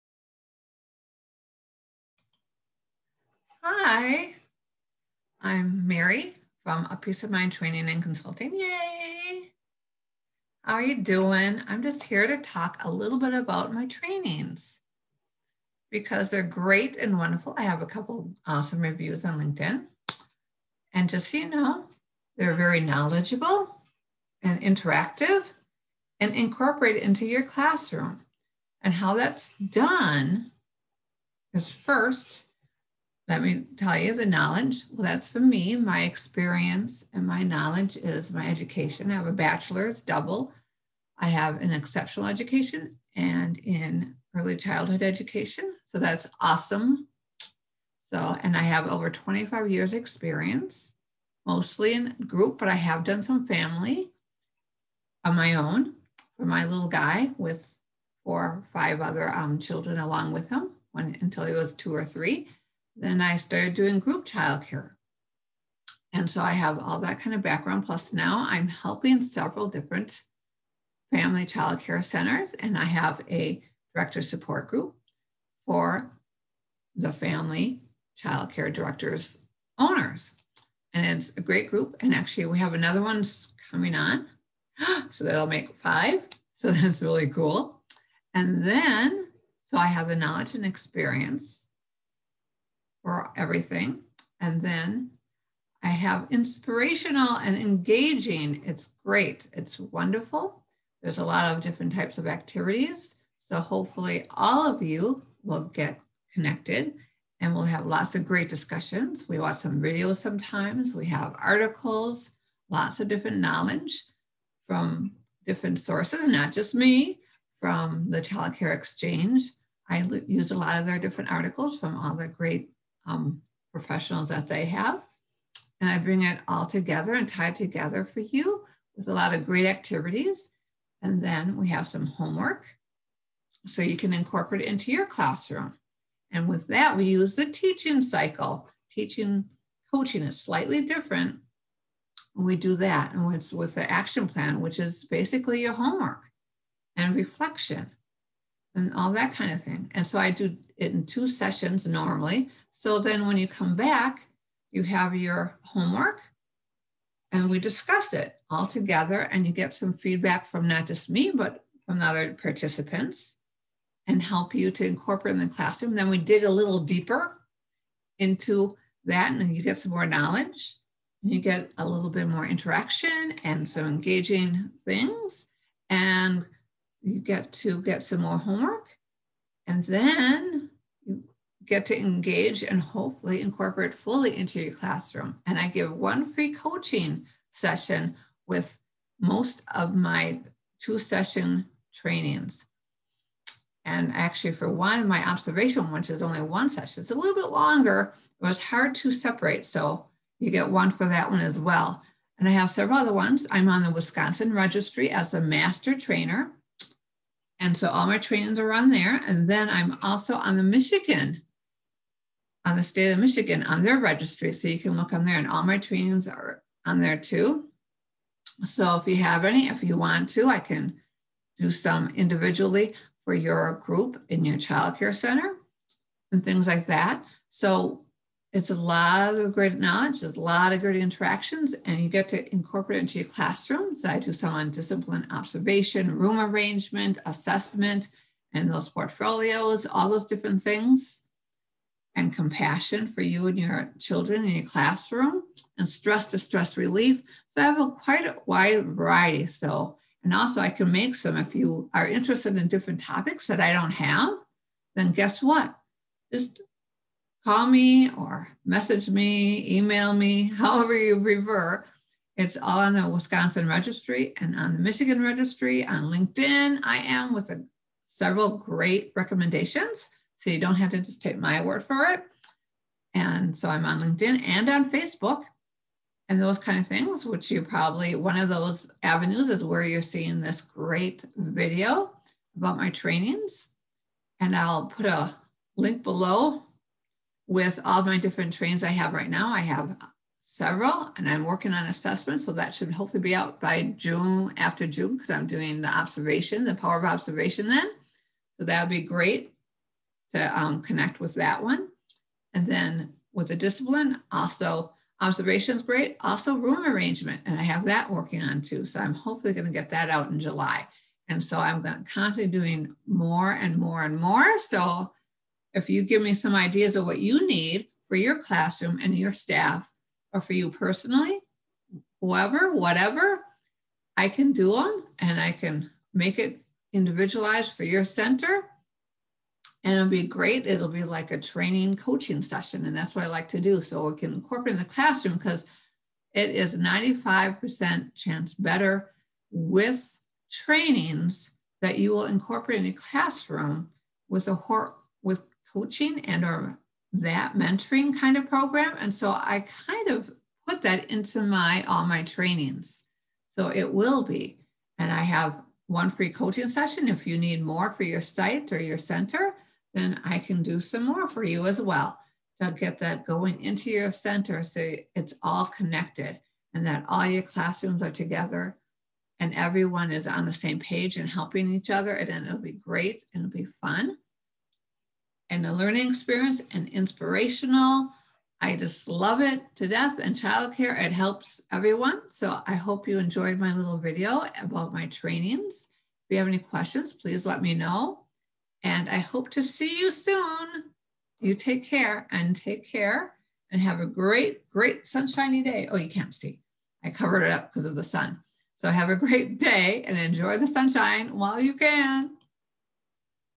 Engaging Childcare Trainings
My engaging childcare trainings take both the teaching and coaching cycle and put them together in away that helps you bring the training back to your classroom.
First with knowledge and engaging activities.
Then homework, the start of your action plan.
Then another session with reflection and discussion.
All two session trainings comes with one coaching session.
Most trainings are two part to better help with incorporating the knowledge back into your classroom.